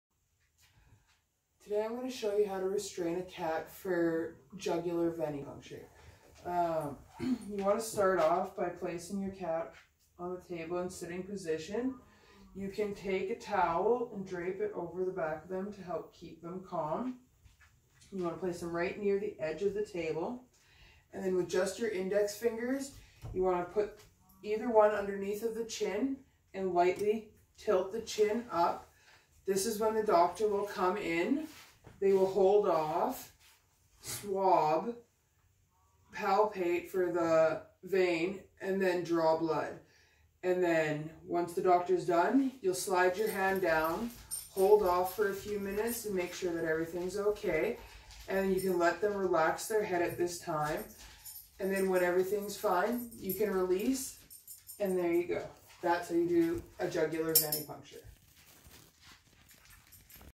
Sternal recumbency jugular venipuncture
How to restrain a cat in sternal recumbency or sitting position for a jugular blood draw.